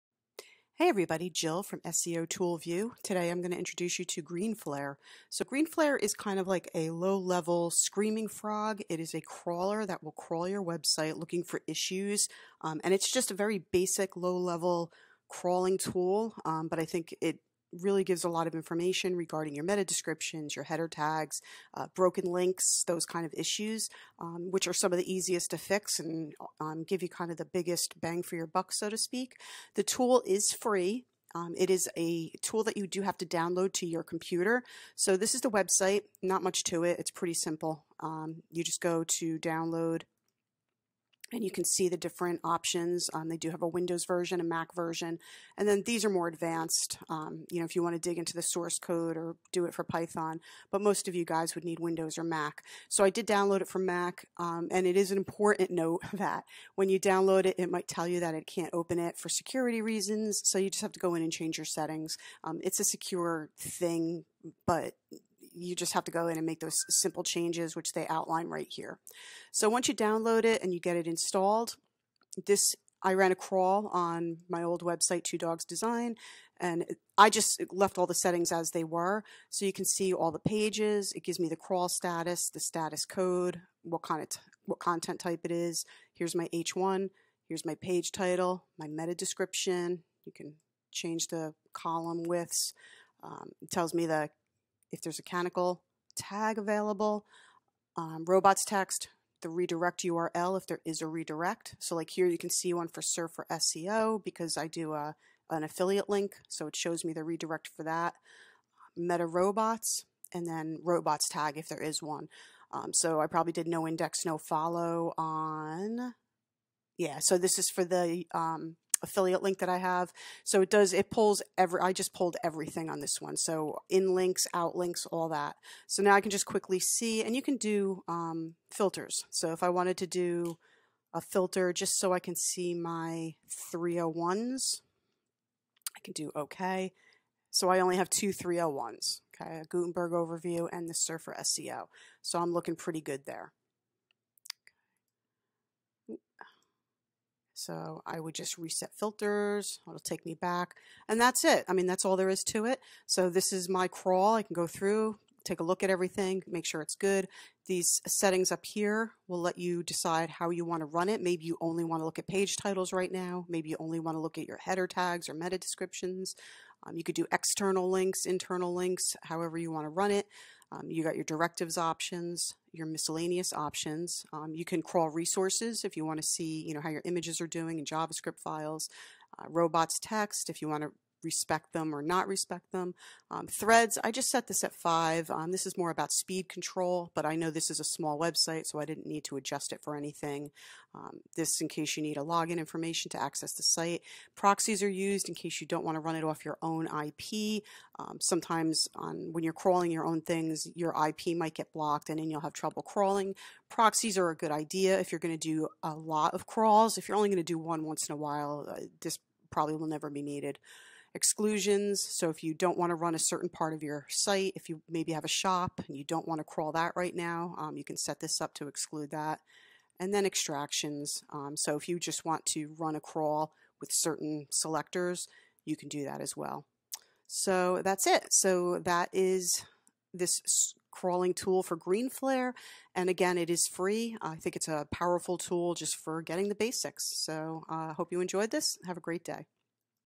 Greenflare Web Crawler Overview
Greenflare is a free - yes free - SEO analysis tool. Check out my video and see what it can do and how it can help you find issues to help you improve your onsite SEO.

Greenflare features include:

Crawl Status
Status Code Errors
Broken Links
Robots status
View H1 and H2 headers
Meta Descriptions

You can also use extractions and exclusions to filter and custom define what you want to scrape.

Greenflare has no crawl limits and it is a downloadable software that runs on your computer. Available for Windows, macOS and Linux.